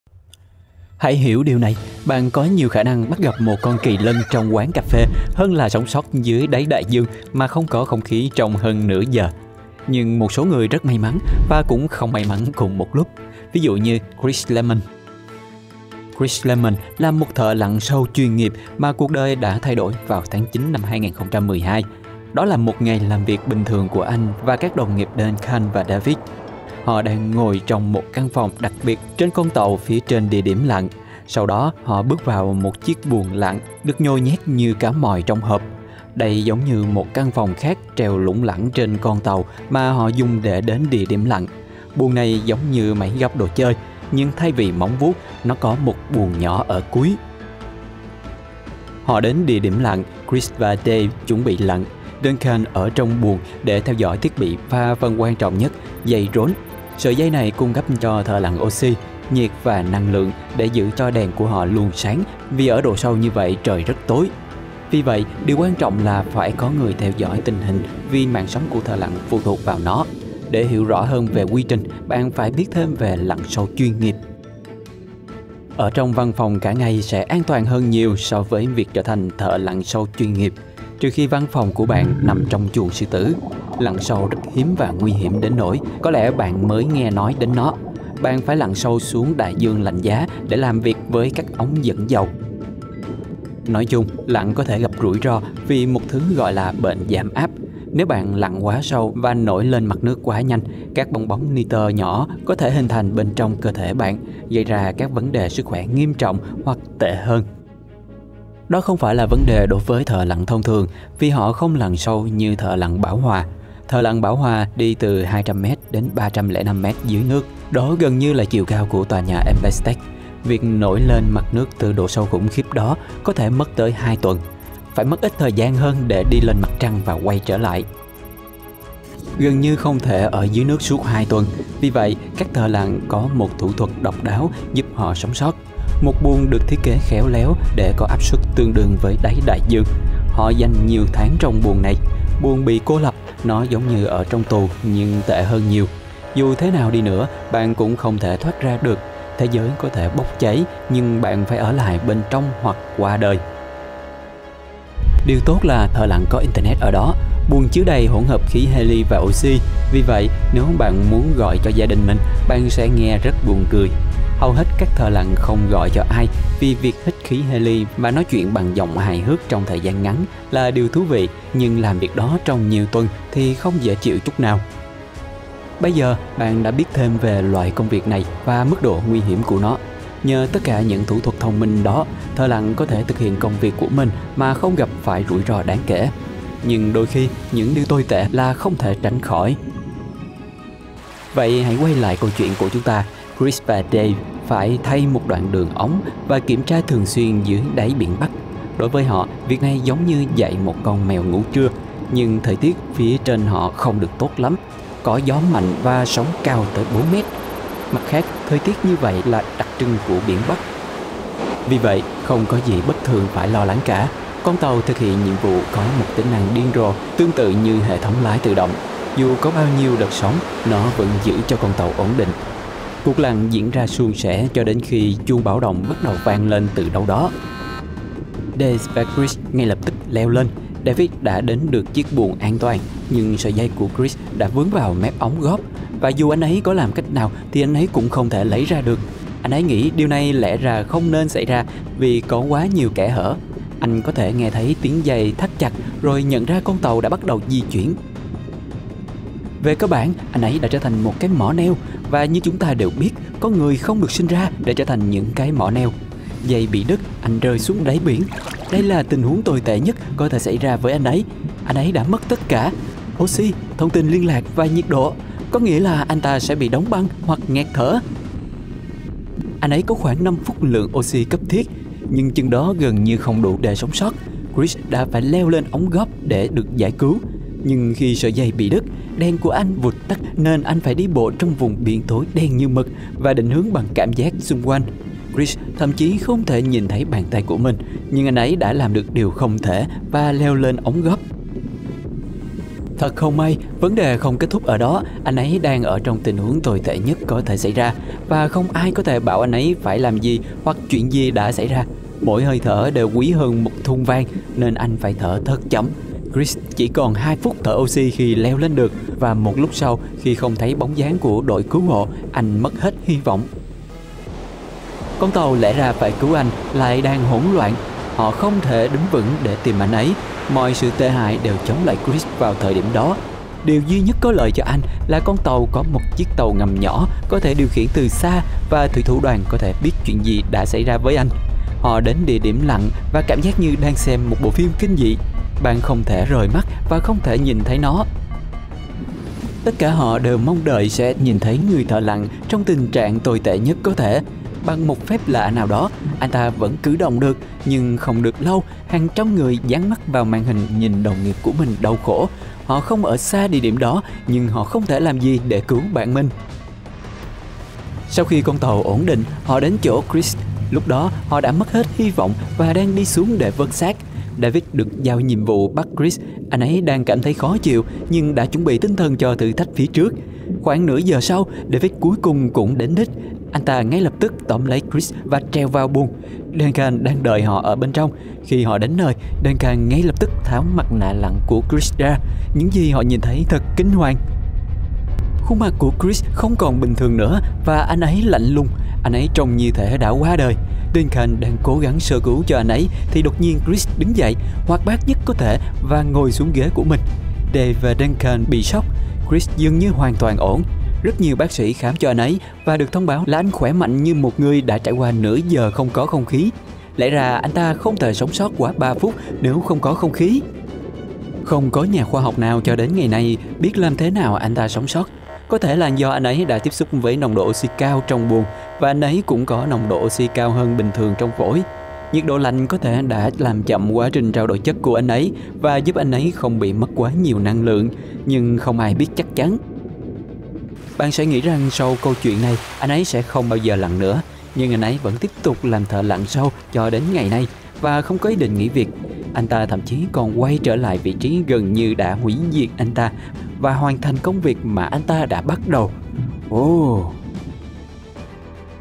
Người đàn ông sống sót sau 30 phút không có oxy
Có một người đàn ông Anh tên là Chris Lemons nhé? Về cơ bản, anh ấy đã trở thành huyền thoại vì anh ấy đã thực hiện được màn sinh tồn điên rồ này. Hãy tưởng tượng thế này: anh ấy đang lạnh cóng dưới đáy Biển Bắc trong hơn 30 phút, không có luồng không khí nào, chẳng có gì. Nhưng hiểu được điều này, bất chấp mọi khó khăn, anh ấy đang sống dở chết dở! Như là về một tình huống thập tử nhất sinh? Chua chát? Đó là ddiefu của những giấc mơ sinh tồn. Làm thế nào nó thậm chí có thể? Chúng ta hãy đi sâu vào để tìm hiểu.